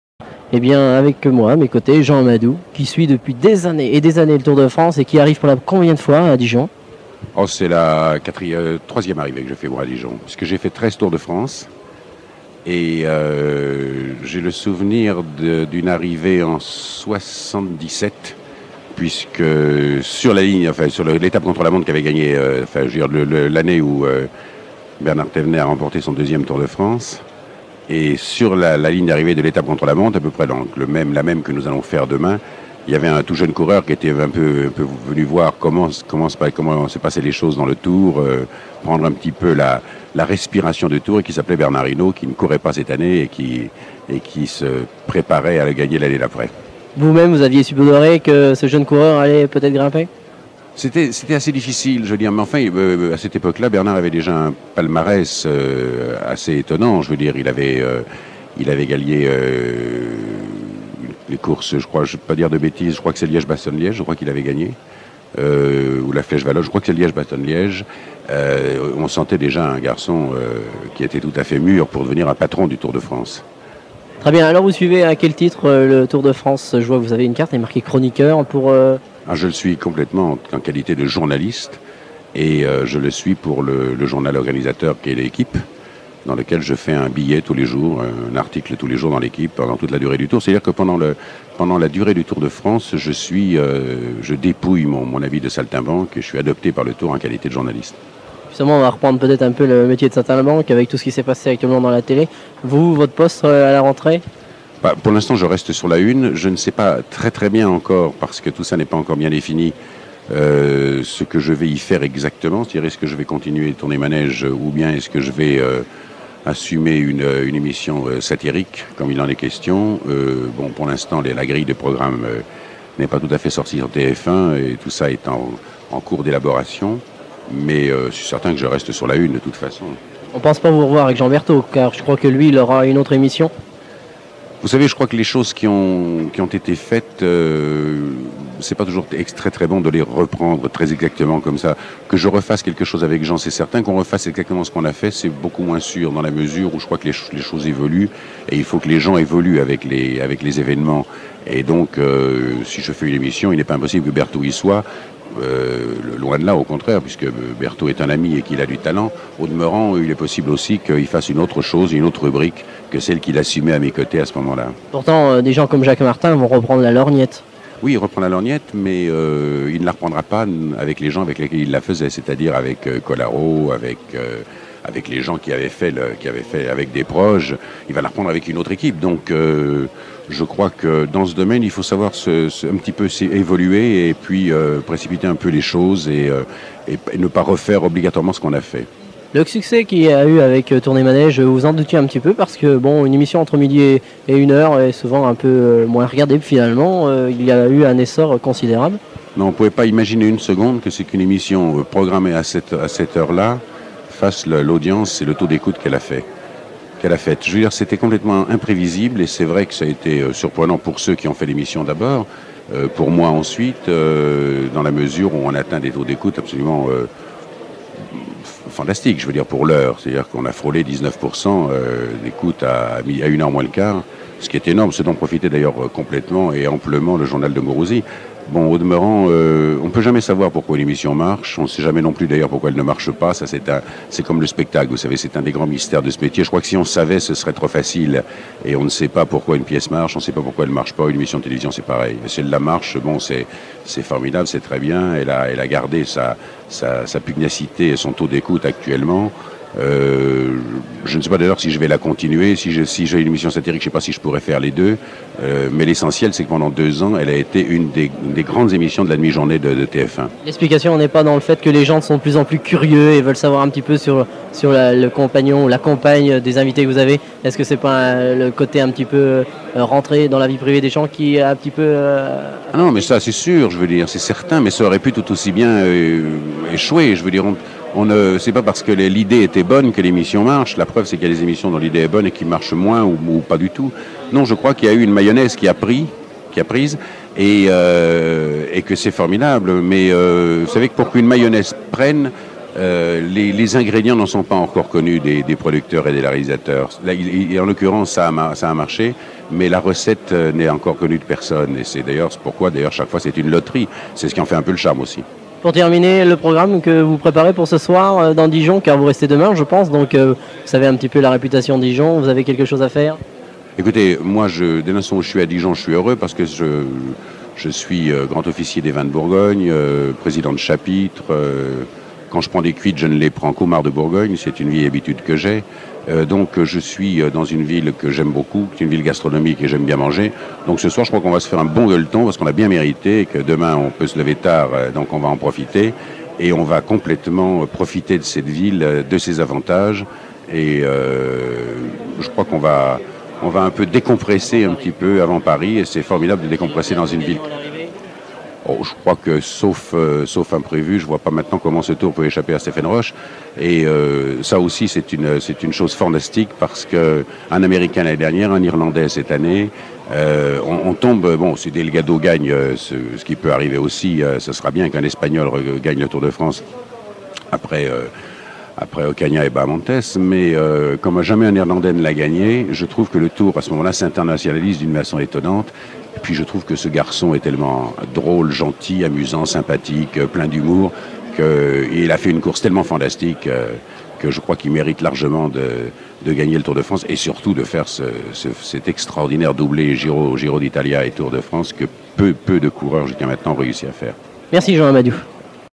JEAN AMADOU , TOUR DE FRANCE 1987 , DIJON
humoriste genial , Jean Amadou était aussi un passionne du Tour De France . J, ai eu de la chance de le rencontrer en juillet 1987 lors du contre la montre de Dijon . Nous sommes au Palais des Sports transforme en salle de presse .